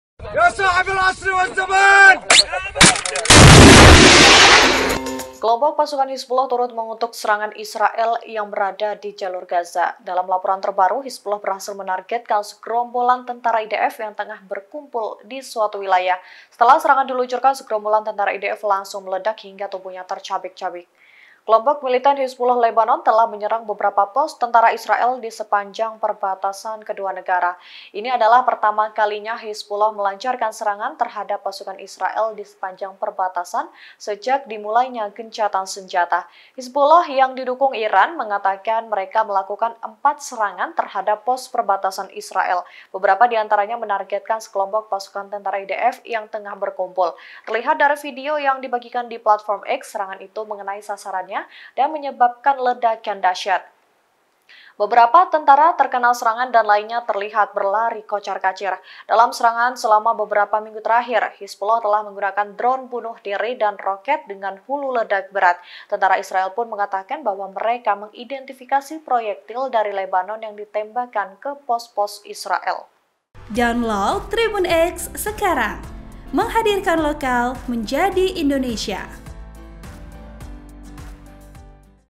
TEPAT SASARAN! Hizbullah Tembak Segerombolan Tentara Israel Jarak Dekat, IDF Lari Kocar-Kacir
TRIBUN-VIDEO.COM- Kelompok pasukan Hizbullah turut mengutuk serangan Israel di Gaza.

Dalam laporan terbaru, Hizbullah berhasil menargetkan segereombolan tentara IDF yang tengah berkumpul di suatu wilayah.

Vp: Dharma